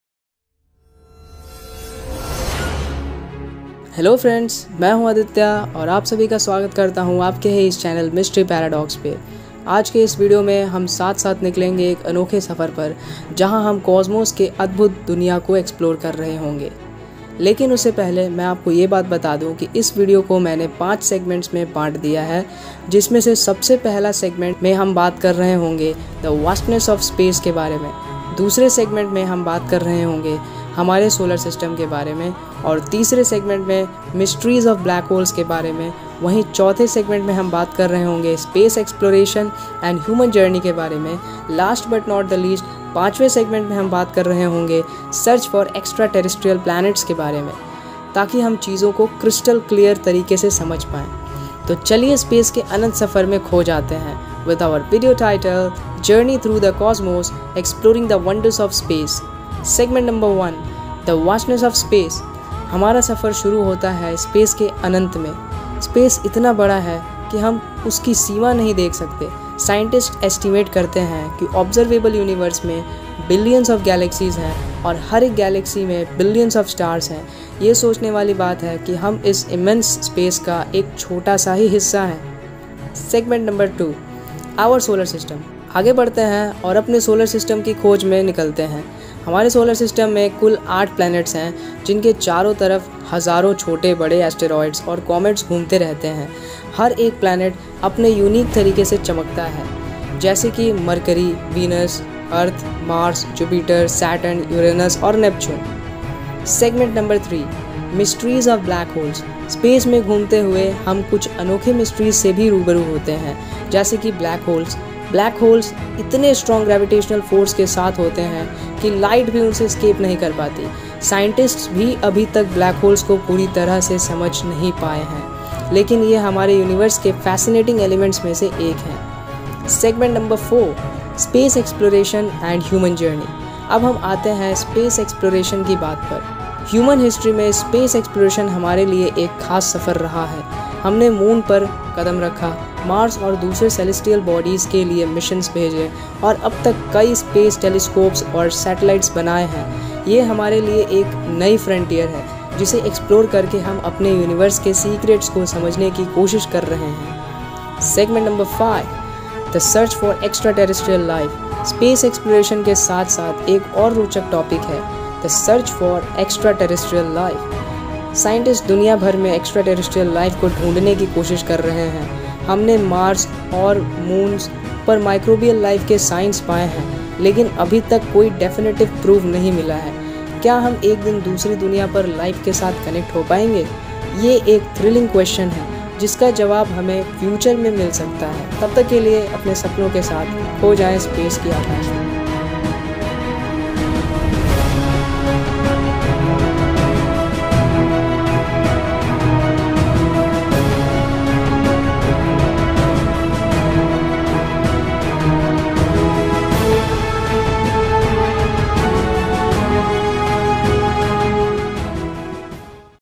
ब्रह्माण्ड से jure 5 रोचक facts|| Universe related 5 Interesting facts
Introduction:
Host (You): Hello friends , main hun aditya aur aap sabhi ka swagat karta hoon aapke hi iss channel "Mystery Paradox"par. Aaj ke iss video mein hum saath-saath niklenge ek anokhe safar par, jahan hum cosmos ke adbhut duniya ko explore karenge.

Lekin usse pahle main apko ye bata dun ki iss video ko maine 5 segments mein baat diya hai jismein se sabse pahle segment mein hum baat kar rahe honge The vastness of space ke baare mein. dusre segment mein hum baat kar rahe honge humare solar system ke baare mein...aur teesre segment mein mysteries of black holes ke baare mein. wahi chauthe segment mein hum baat kar rahe honge space exploration and human journey ke bare mein .last but not the least pachwe segment mein hum baat kar rahe honge search for extraterrestrial planets ke bare mein. taki hum chijo ko crystal clear tarike se samajh paye.

Toh chaliye, space ke anant safar mein kho jate hain.
With our video title "Journey Through the Cosmos: Exploring the Wonders of Space"

Segment 1: The Vastness of Space
Host: Hamara safar shuru hota hai space ke anant mein. Space itna bada hai ki hum uski seema nahi dekh sakte. Scientists estimate karte hain ki observable universe mein billions of galaxies hain, aur har ek galaxy mein billions of stars hain. Yeh sochne wali baat hai ki hum is immense space ka ek chhota sa hissa hain.

Segment 2: Our Solar System
Host: Aage badhte hain aur apne Solar System ki khoj mein nikalte hain. Hamare Solar System mein 8 planets hain, jinke charon taraf hazaaron chhote-bade asteroids aur comets ghoomte rehte hain. Har ek planet apne unique tareeke se chamakte hai. Jaise ki Mercury, Venus, Earth, Mars, Jupiter, Saturn, Uranus aur Neptune.

Segment 3: Mysteries of Black Holes
Host: Space mein ghoomte hue hum kuchh anokhe mysteries se bhi rubaru hote hain, jaise ki Black Holes. Black Holes itne strong gravitational force ke saath hote hain ki light bhi unse escape nahi kar pati. Scientists bhi abhi tak Black Holes ko puri tarah se samajh nahi paye hain, lekin yeh humare universe ke fascinating elements mein se ek hain.

Segment 4: Space Exploration and Human Journey
Host: Ab hum aate hain space exploration ki baat par. Human history mein space exploration humare liye ek khaas safar raha hai. Humne Moon par kadam rakha, Mars aur dusre celestial bodies ke liye missions bheje, aur ab tak kei space telescopes aur satellites banaaye hain. Yeh humare liye ek nayi frontier hai, jise explore karke hum apne universe ke secrets ko samajhne ki koshish kar rahe hain.

Segment 5: The Search for Extraterrestrial Life
Host: Space exploration ke saath-saath ek aur rochak topic hai "The Search for Extraterrestrial Life." Scientists duniya bhar mein extraterrestrial life ko dhoondne ki koshish kar rahe hain. Humne Mars aur kuchh moons par microbial life ke signs paye hain, lekin abhi tak koi definitive proof nahi mila hai. Kya hum ek din dusri duniyaon par life ke saath connect ho payenge? Yeh ek thrilling question hai, jiska jawab hume future mein mil sakta hai.

Conclusion:
Host: Toh dosto, yeh tha hamara safar space ke adbhut duniya mein. Space ki khoj humare liye ek anokha aur challenging kaam hai, lekin yeh hume apne existence aur universe ke bare mein naye raaz kholene mein madad karti hai. Milte hain agli video mein, tab tak ke liye, apne sapno ke sath kho jaiye space ke aakash mein!